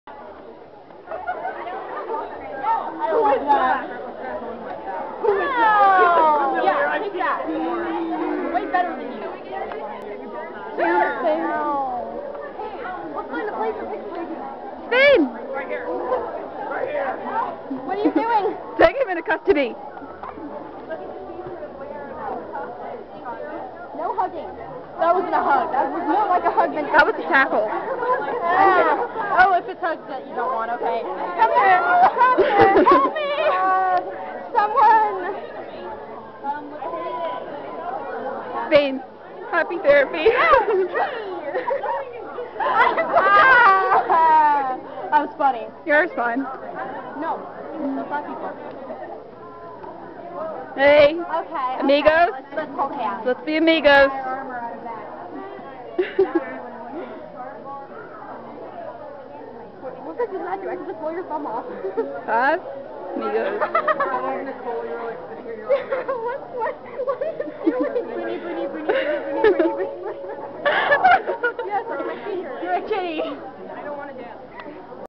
KuroNeko con Hetalia - Spain Vs. America
Bunibunibunibunibunibuni~

me and X0NightSky0X and our amigos at KuroNeko con 2011